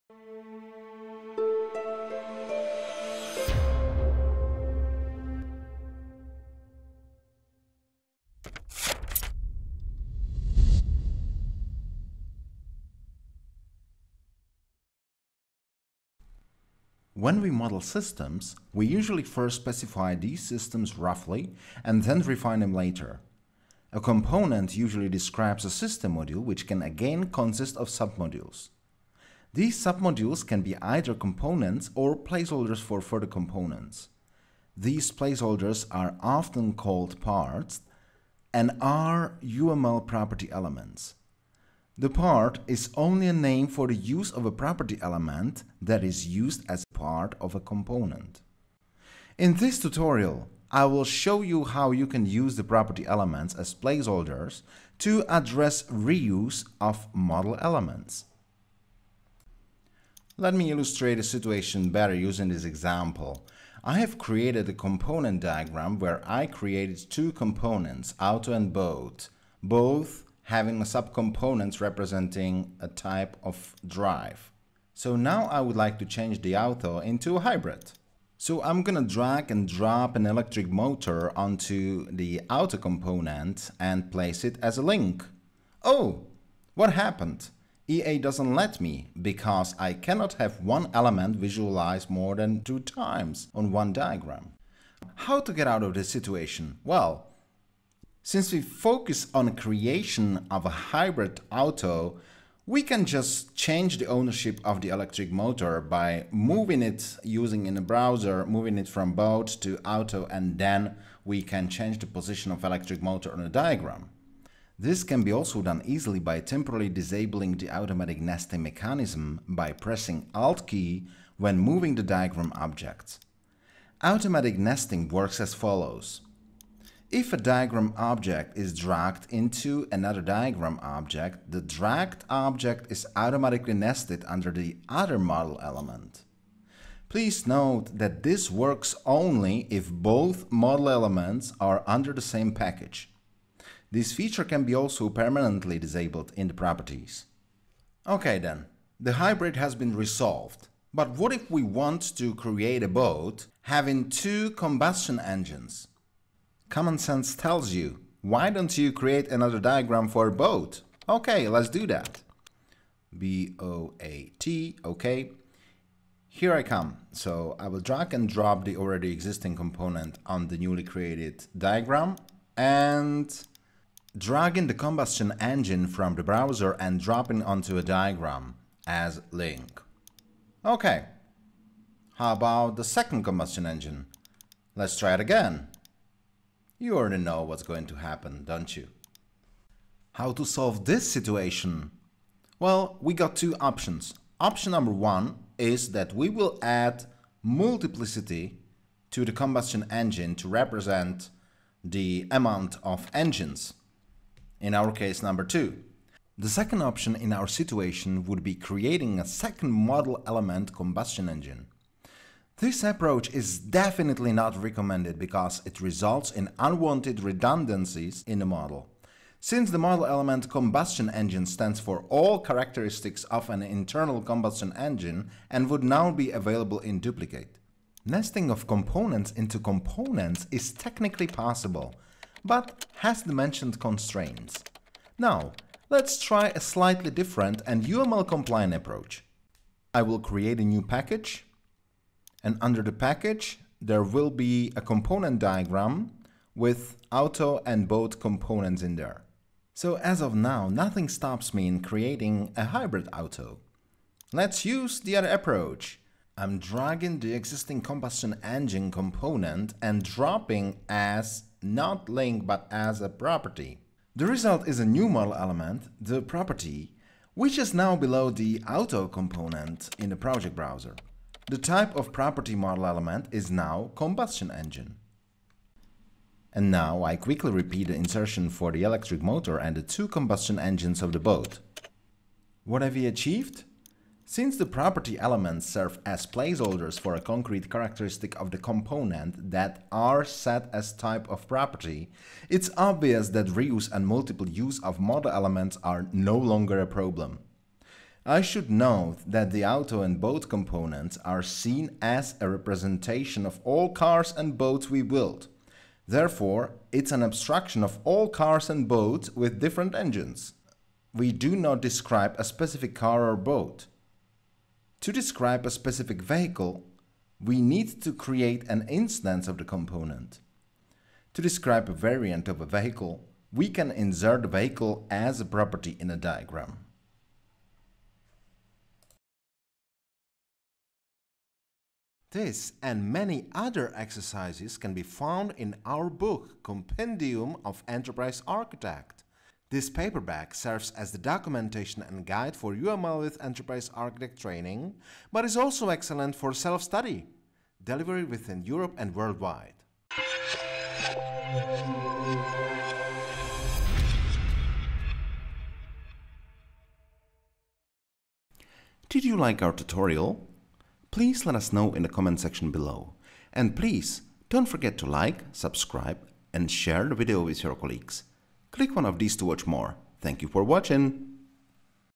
Practical use of property elements
When we model systems, we usually first specify these systems roughly and then refine them later. A component usually describes a system module which can again consists of sub-modules. These sub-modules can be either components or placeholders for further components. These placeholders are often called Parts and are UML property elements. The Part is only a name for the use of a property element that is used as part of a component.
In this tutorial I will show how you can use the property elements as placeholders to address reuse of model elements.